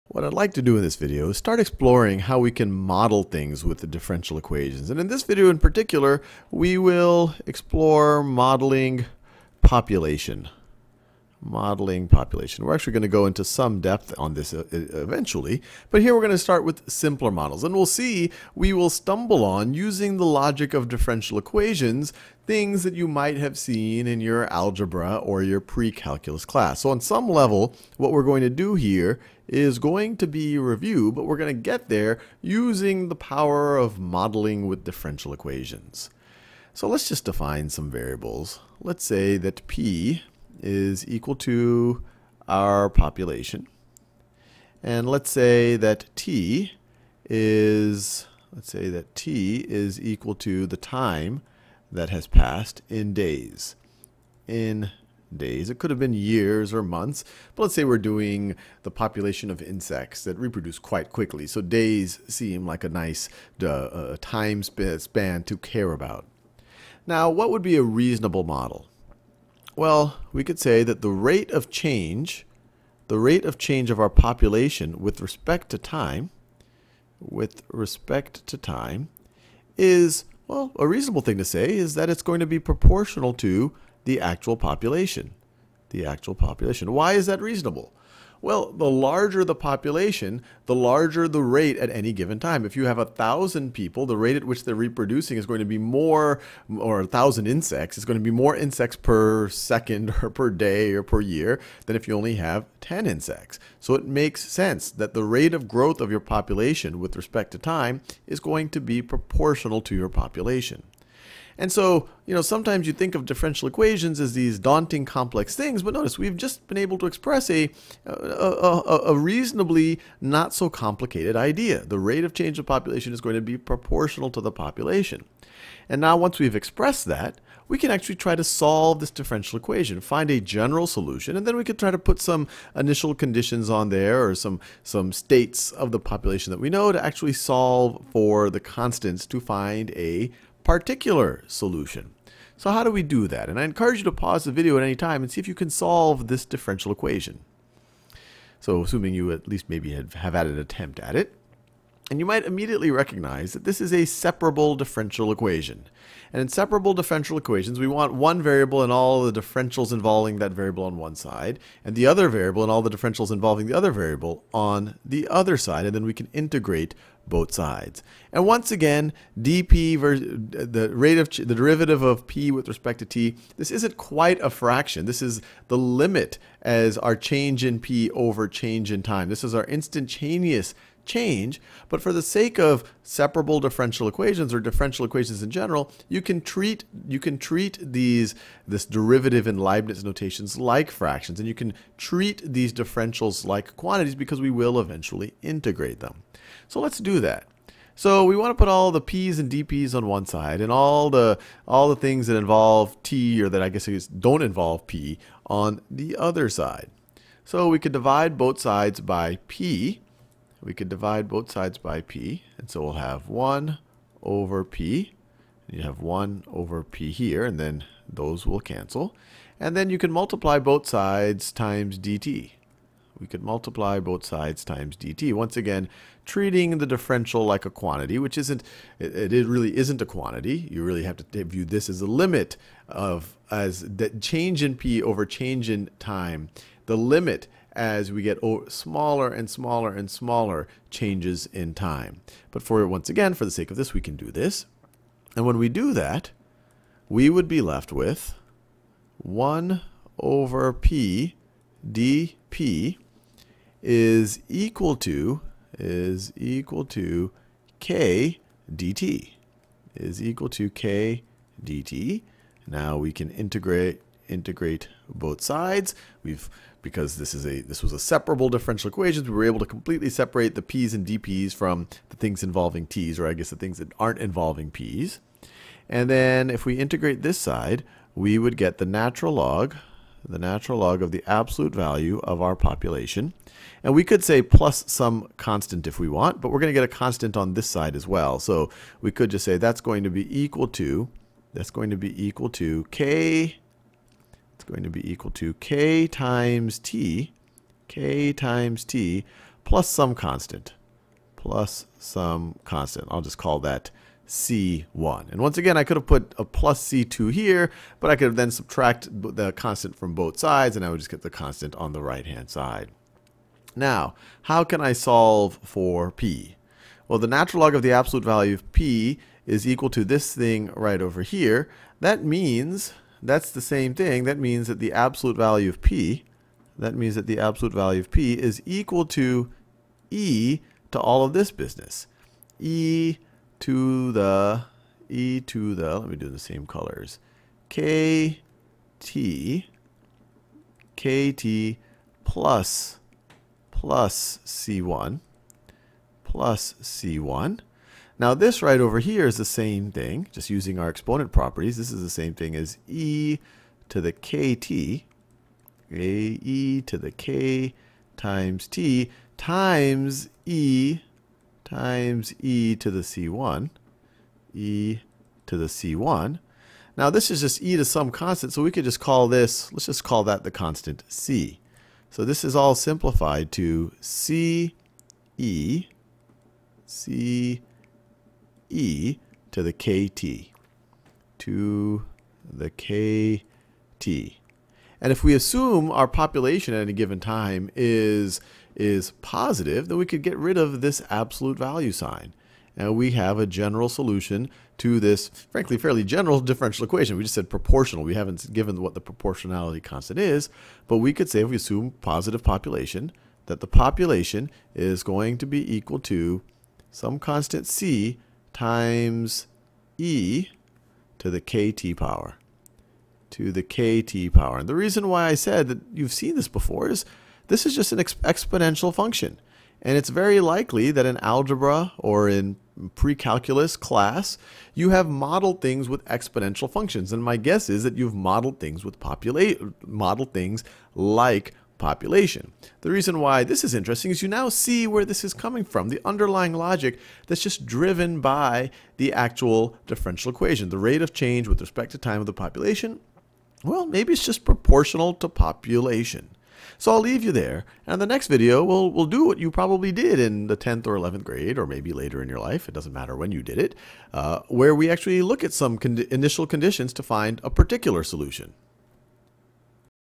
Modeling population with simple differential equation | Khan Academy
Another separable differential equation example.

Differential Equations on Khan Academy: Differential equations, separable equations, exact equations, integrating factors, homogeneous equations.